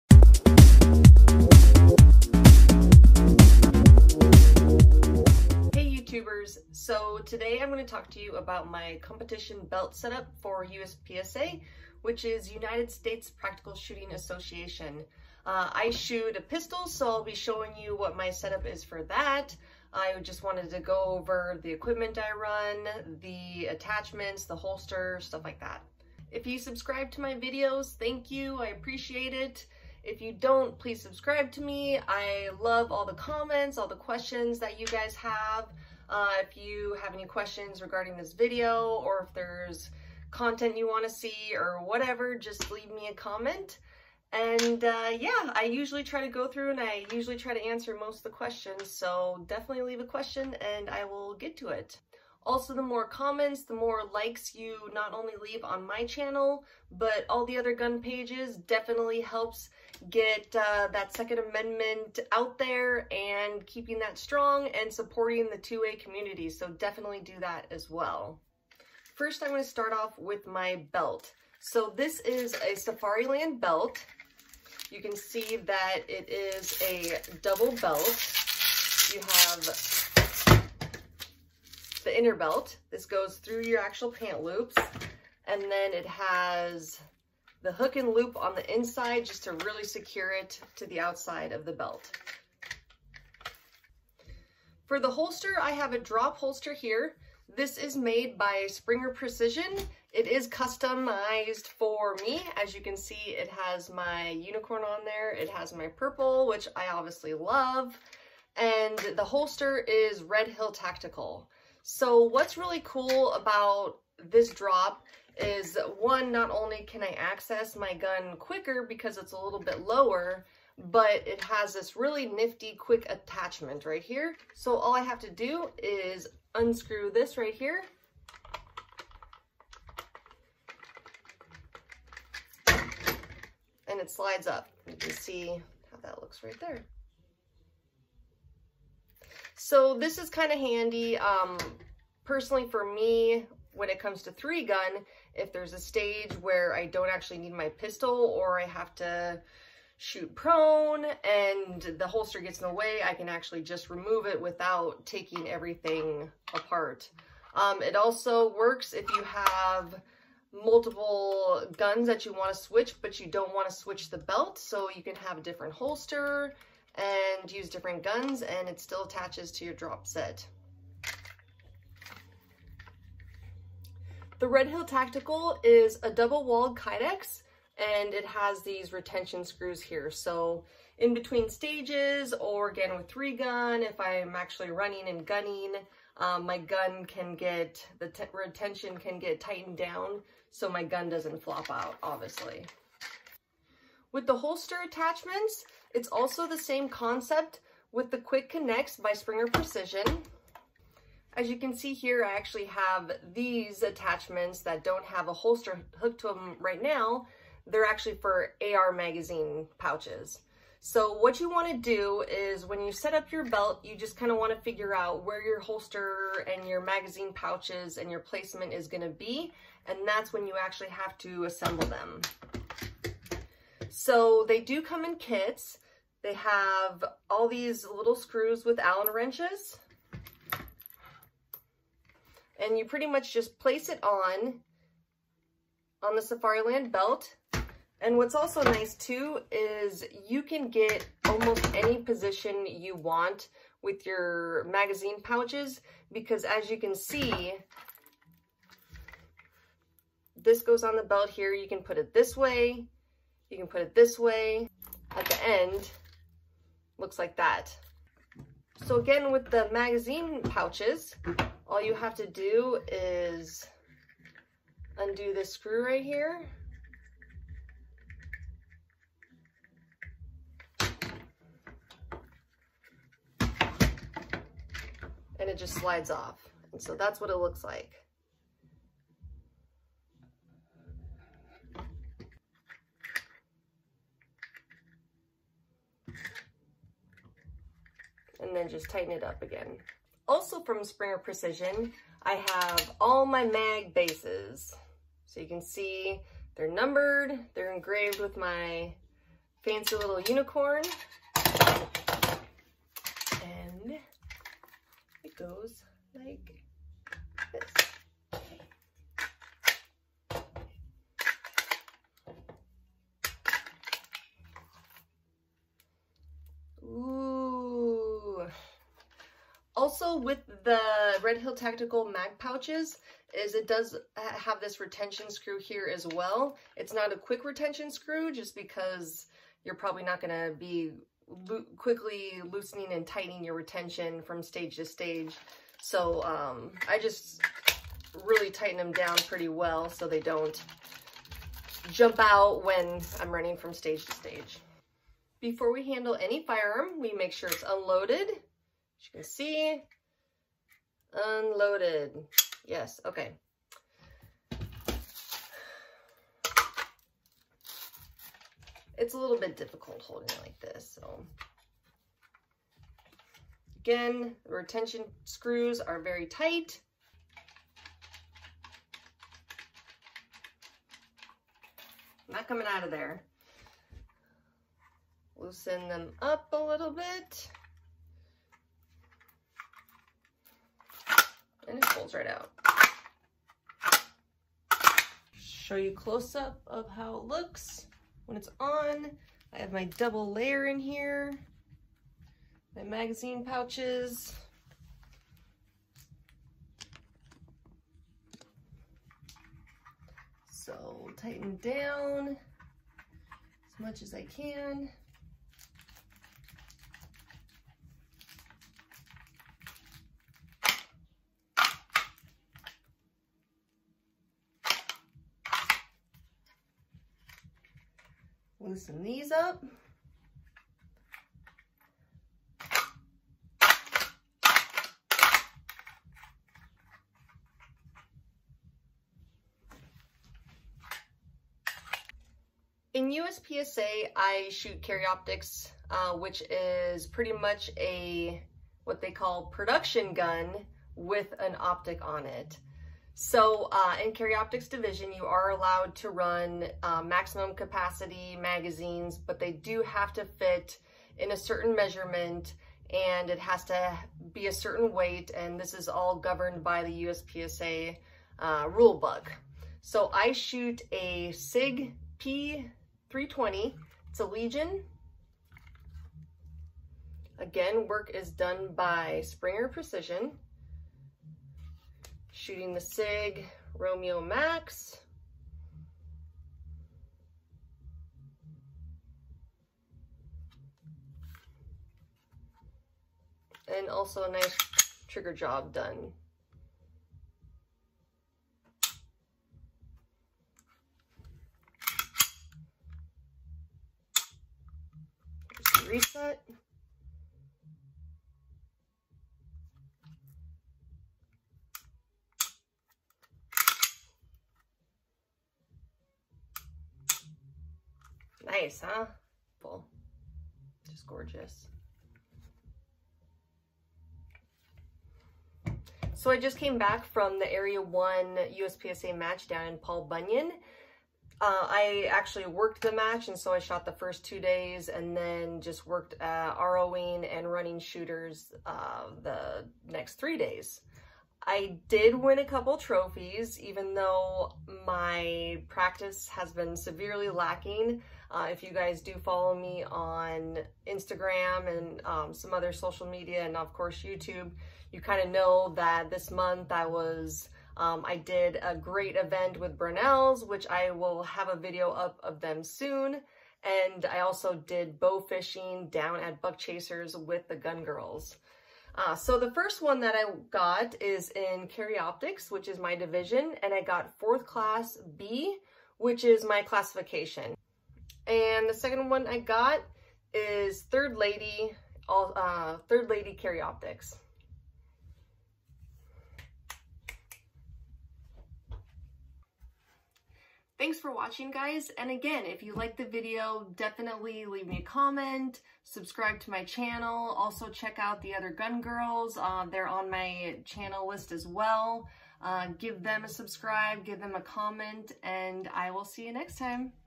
Competition Belt Set Up
I go through my competition belt set up for USPSA in Carry Optics Division. Recently I came back from the USPSA Area 1 Championship and won 4th place B class in Carry Optics out of 39 and 3rd place Lady in Carry Optics out of 11. Carry Optics in the biggest Division and I placed 50th of 131 shooters.
Red Hill Tactical CODE: BRAIDEDUNICORN10
Run and Gun Apparel CODE: BRAIDEDUNICORN10